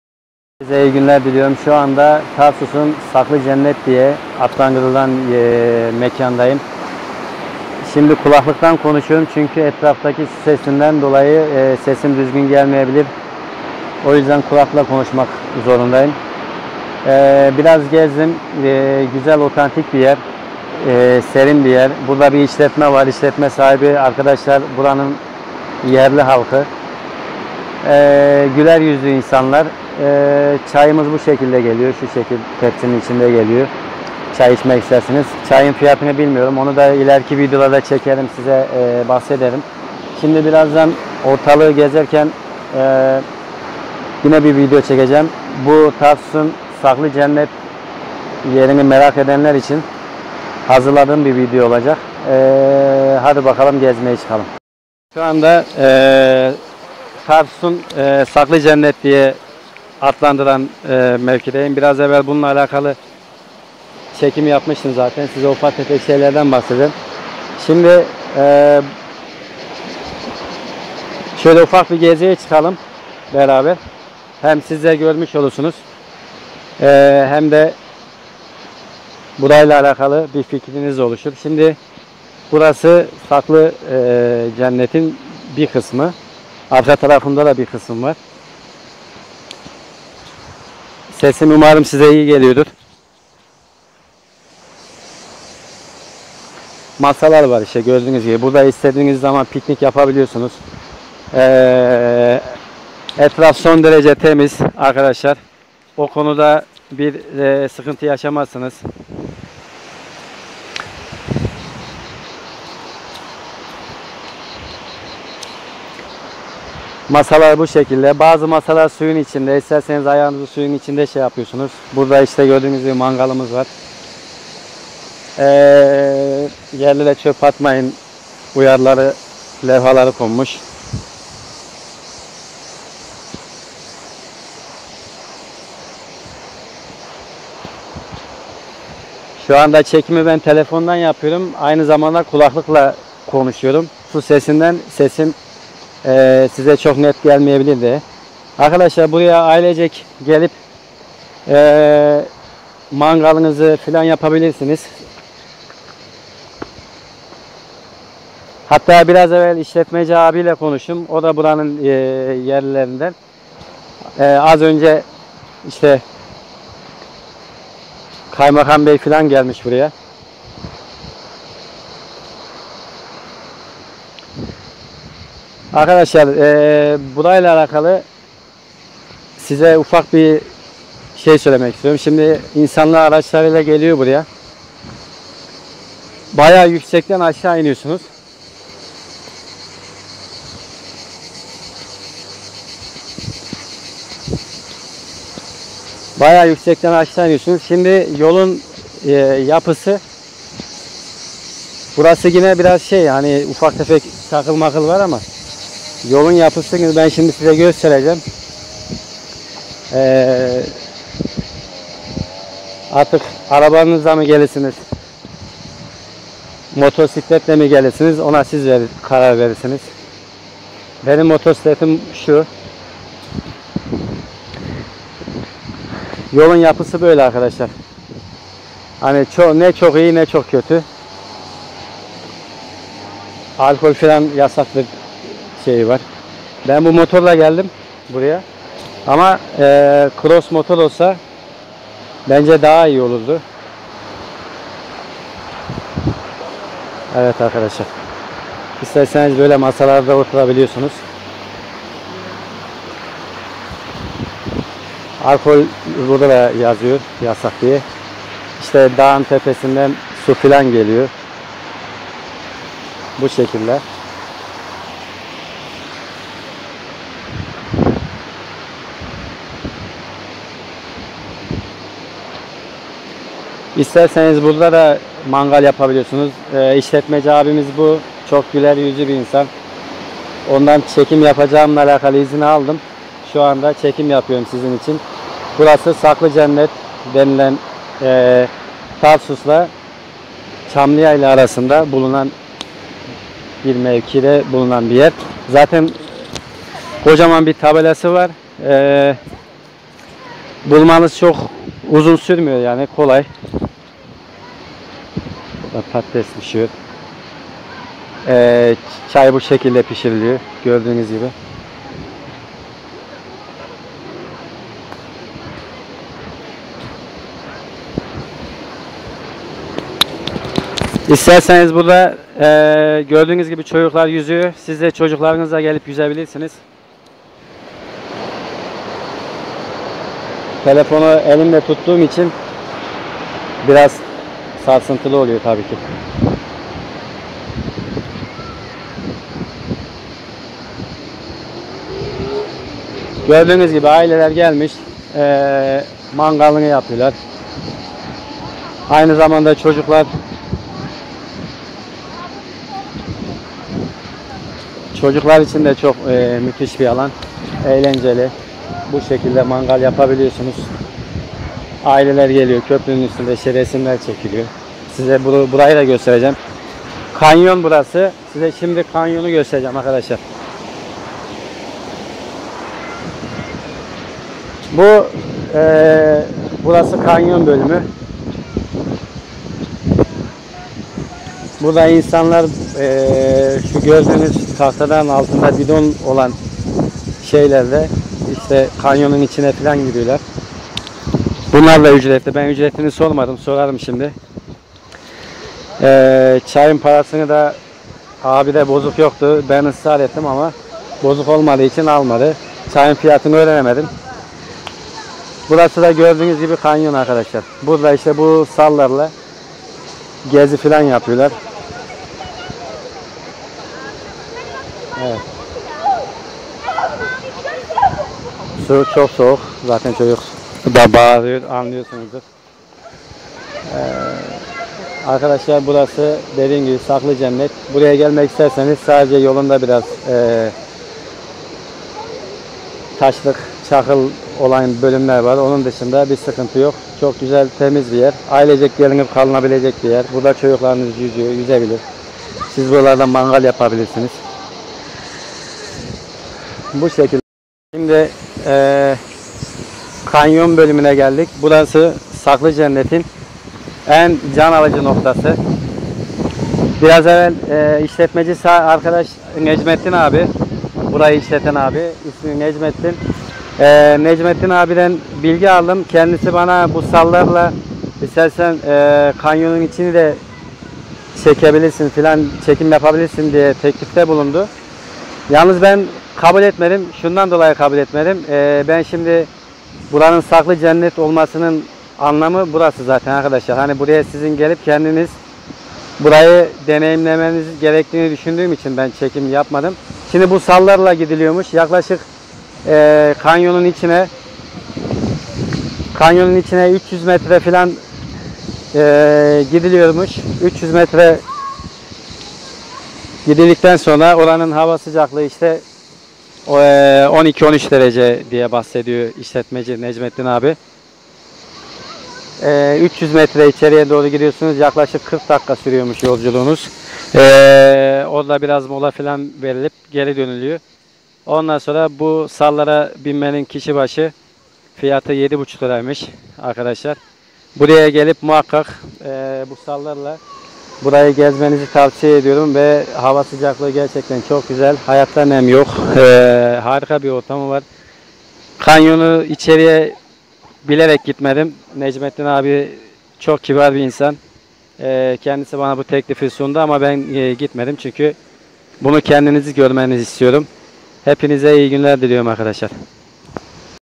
tarsus sarikavak saklıcennet
Tarsus çamliyayla arasında sarikavak mevkiisinde yeralan gizli bir mekan Sakli cennet